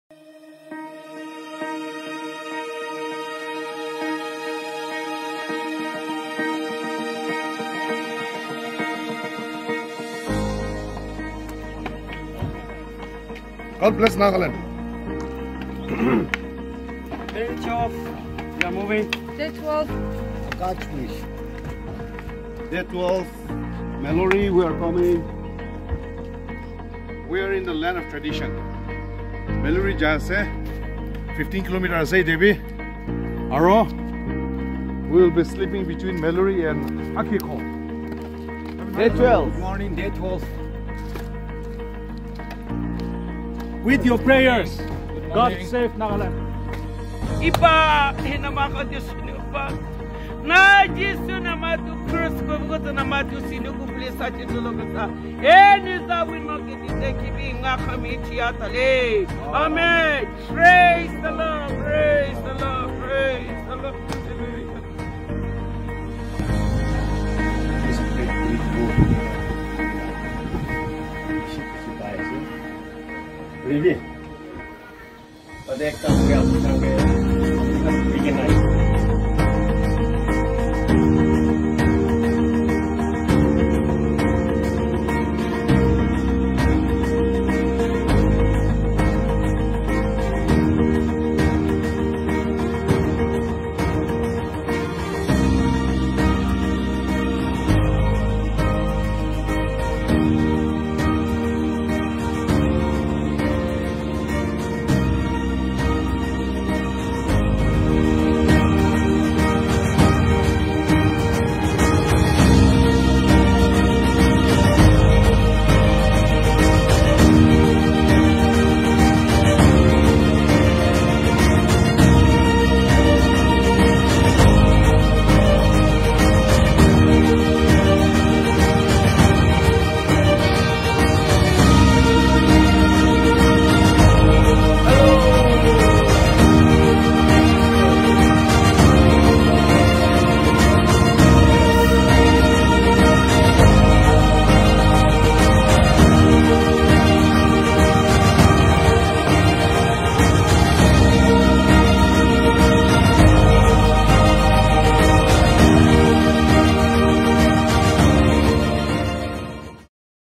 Rising People's Party | Walkathon | A Walk to save Nagaland | Day-12 | Way to Meluri
Day - 12
| Akash bridge to midway Akhwego Village |

The Rising People’s Party (RPP) has started its journey to undertake an awareness campaign under the theme ‘A Walk To Save Nagaland’ from February 21 at 7:30 am from 7th Mile, Chumukedima. The walkathon aims to cover 19 areas and districts of Nagaland. The proposed campaign started its campaign for the next General Assembly Elections in the state. The walk commenced from Dimapur on February 21 and will culminate in Dimapur district itself. The walk will cover a total of 1200 plus kilometres and a total of 40 days or more to cover the distance.
The main motive of the walk is to highlight the actual condition of the district and to share RPP's ideology and vision with the people.